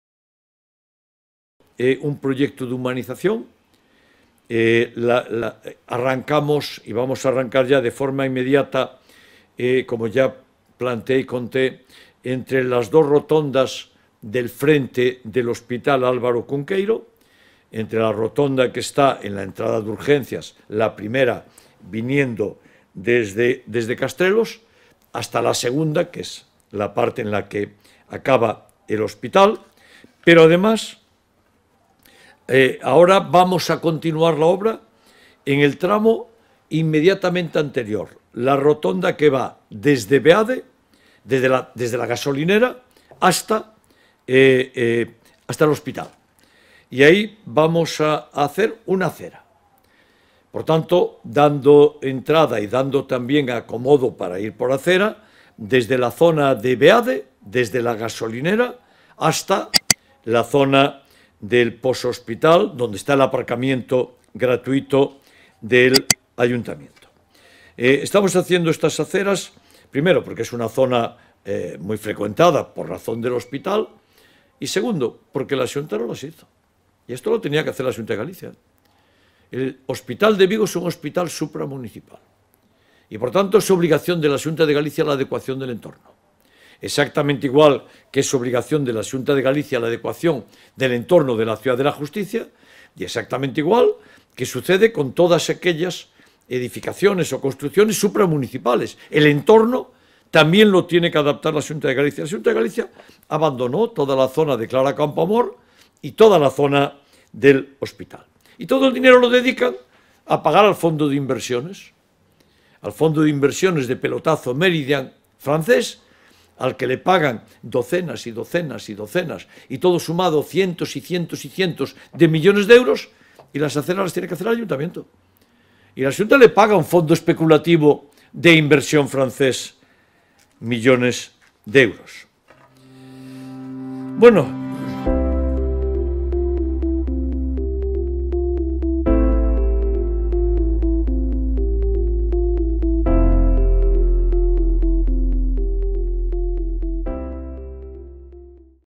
O goberno municipal de Vigo avanza a segunda fase de humanización de Clara Campoamor
22 de outubro de 2021.- O Concello mellorará a seguridade de peóns e vehículos no tramo que comprende entre a rotonda de Beade e a primeira rotonda de acceso ao hospital, logo de ter aprobada a primeira fase que abarca o frontal do Álvaro Cunqueiro.

O alcalde explicou en rolda de prensa que o goberno de Vigo comezará “de forma inmediata” as obras de humanización da primeira fase da Avenida Clara Campoamor, no tramo que vai desde a primeira rotonda de acceso ao hospital vindo desde Castrelos ata a seguinte rotonda, que dá acceso á VG20 e onde remata o hospital.

Adicionalmente, o Concello continuará a obra no tramo anterior, nunha segunda fase de humanización desde a rotonda de Beade (onde está a gasolineira) ata a primeira rotonda de acceso ao hospital. “Imos facer unha beirarrúa desde a gasolineira ata o aparcamento municipal”, onde remata o edificio hospitalario, apuntou Caballero, para facilitar o tránsito peonil ante o incremento da circulación nesa zona.

O rexedor lamentou que “a Xunta que as tiña que facer non as fixo. O hospital é supramunicpal e é obriga da Xunta de Galicia a adecuación da contorna, pero abandonou toda a zona de Clara Campoamor e do hospital. Dedican todo o diñeiro a pagar ao fondo de inversións francés Meridiam”.